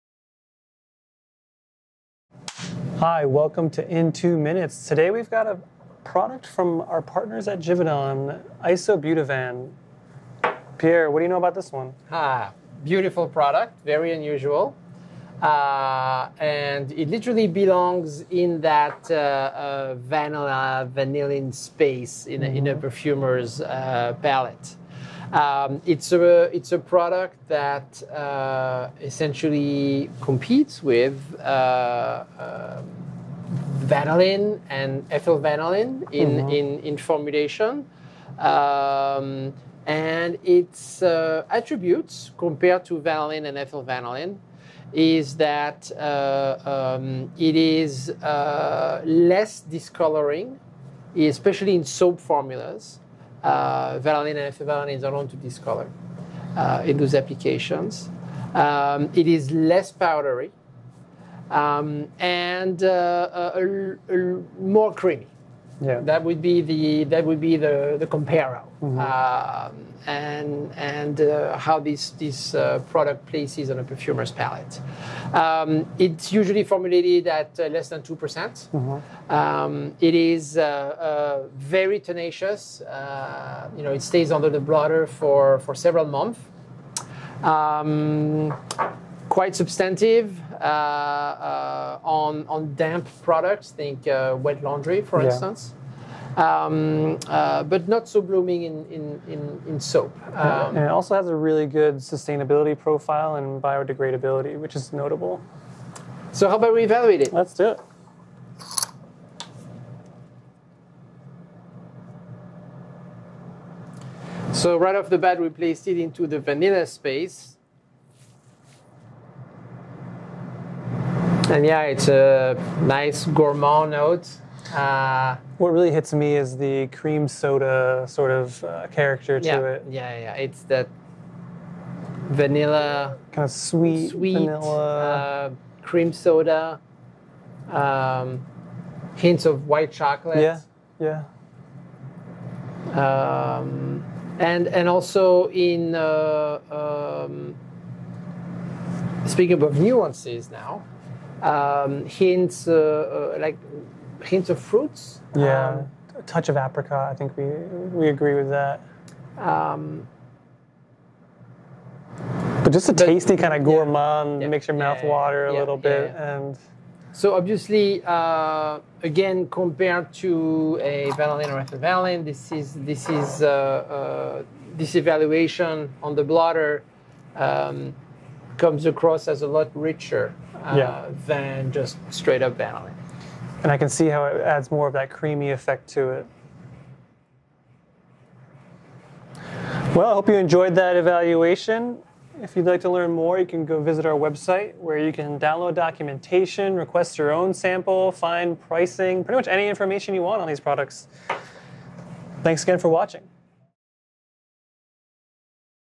Isobutavan: In Two Minutes
Learn more about the Givaudan specialty: Isobutavan (Item #503227) in two minutes.